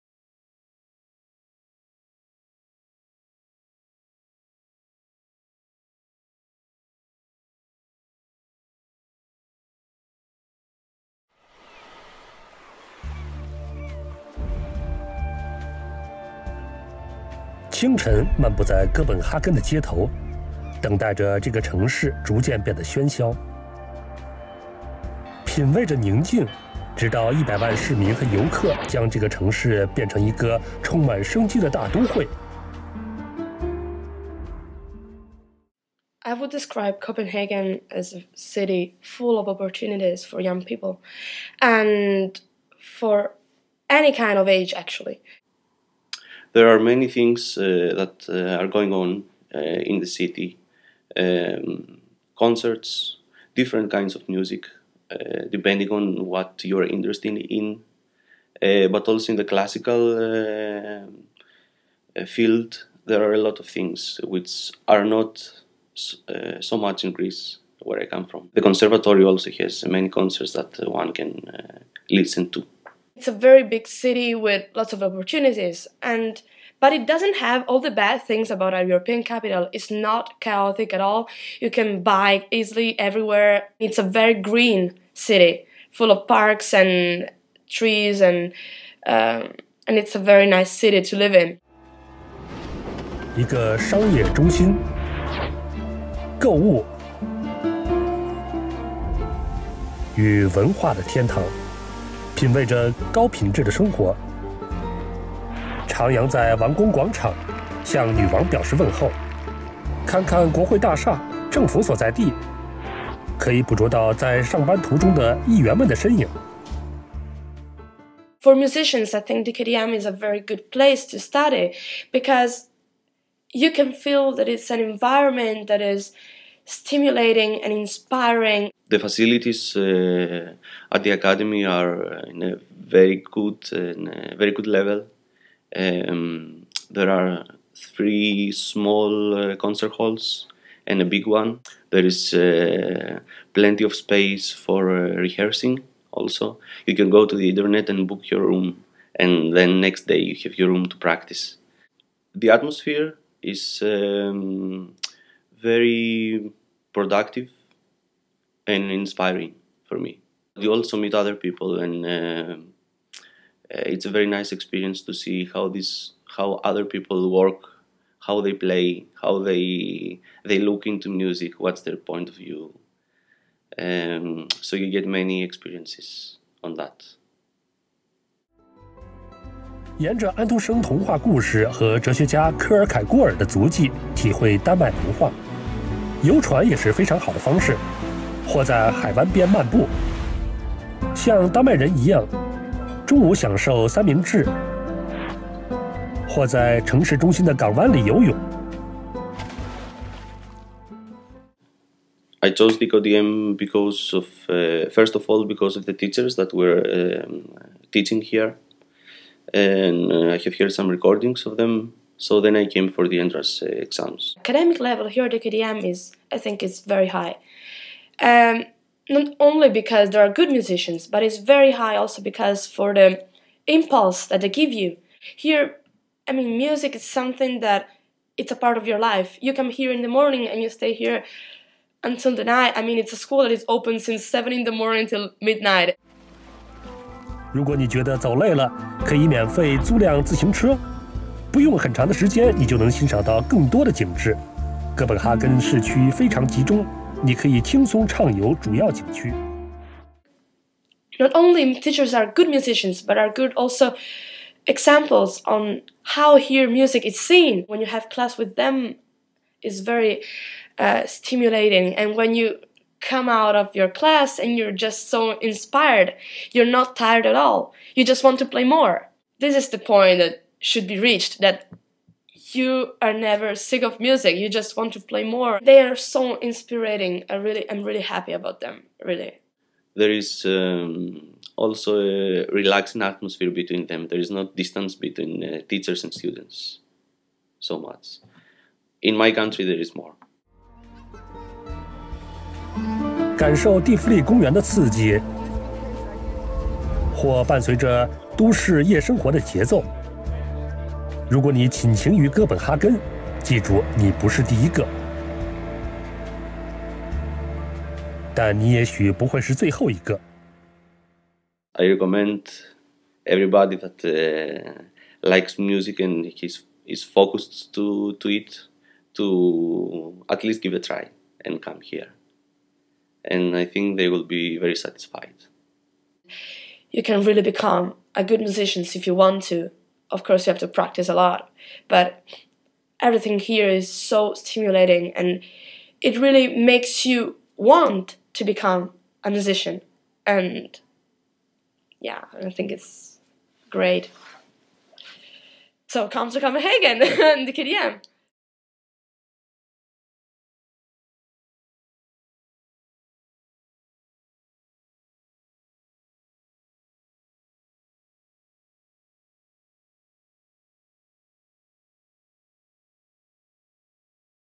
The Royal Danish Academy of Music - student interviews(CH)
International students talking about Copenhagen and studying at DKDM